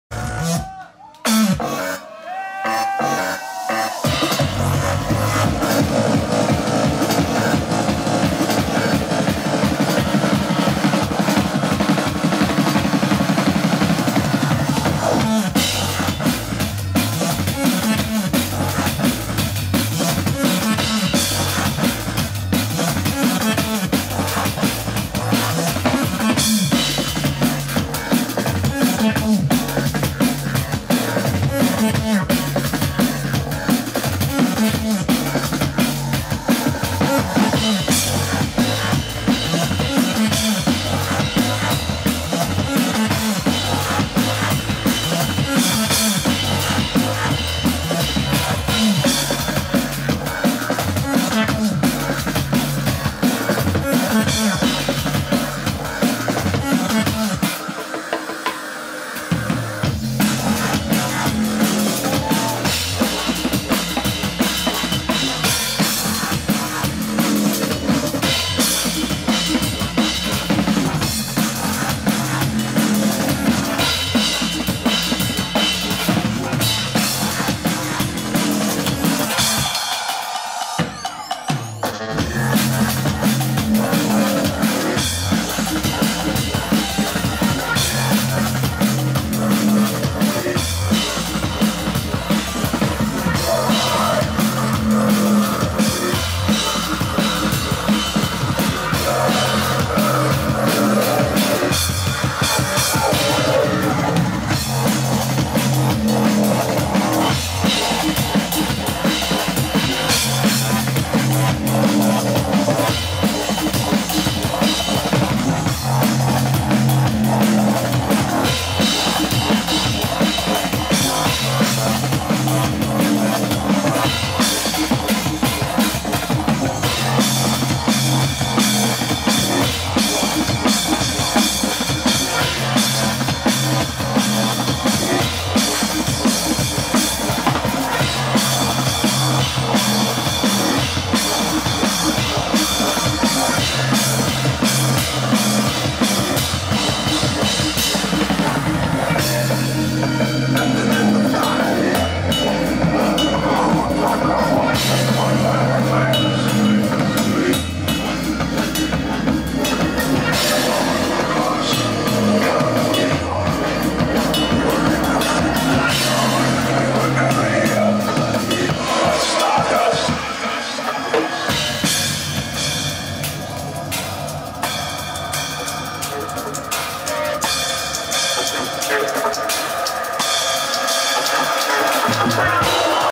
Thom Herian aka Drama Jacqua - Golčfest 2013
Live drum machine Drama Jacqua (Thom Herian) a jeho dnb set na Golčfestu v Golčově Jeníkově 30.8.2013